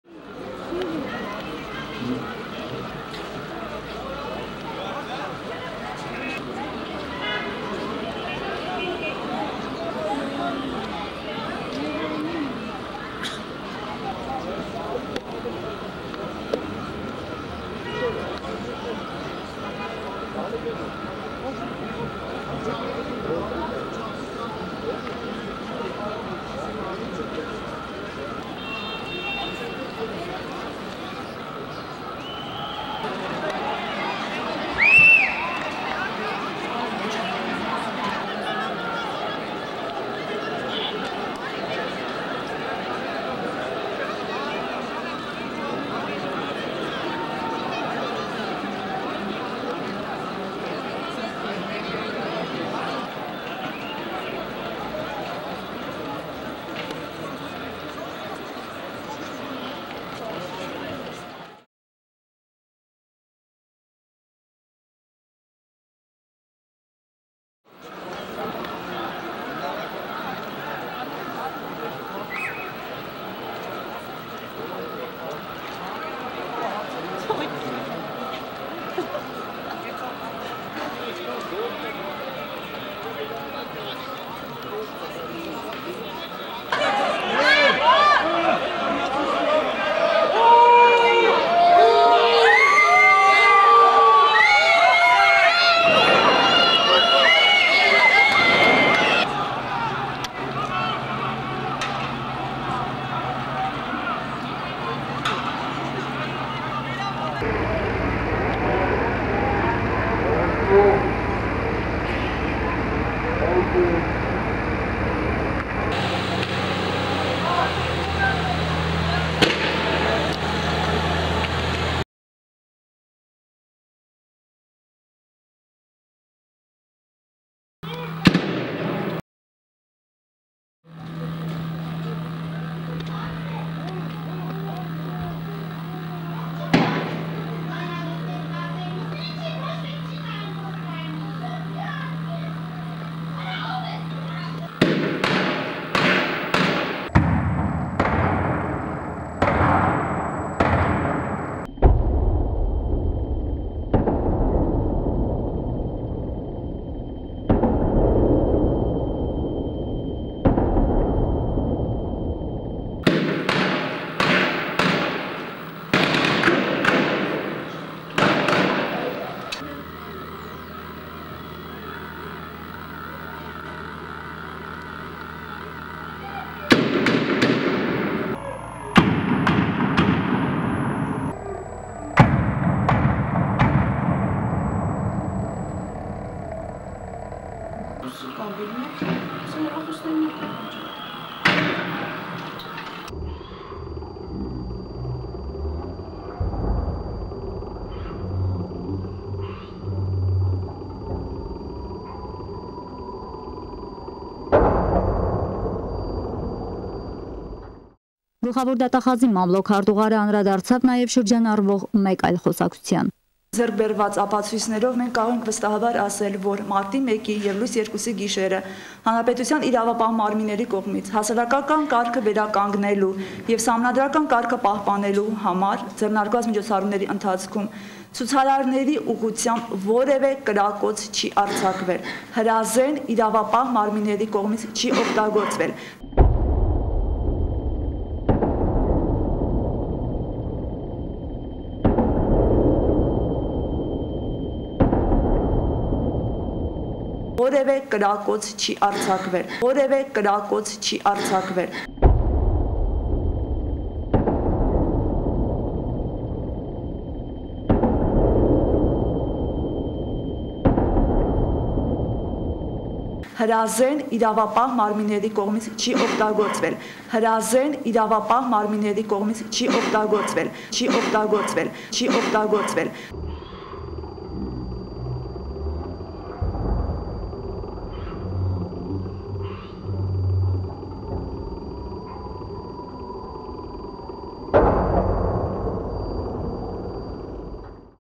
Levon Ter-Petrosyan March 1 2008 shootings
By the order of Kocharyan, Armenian soldiers shoot peaceful Armenian people on the streets who are doing nothing but asking to have fair election in the 21st century Armenia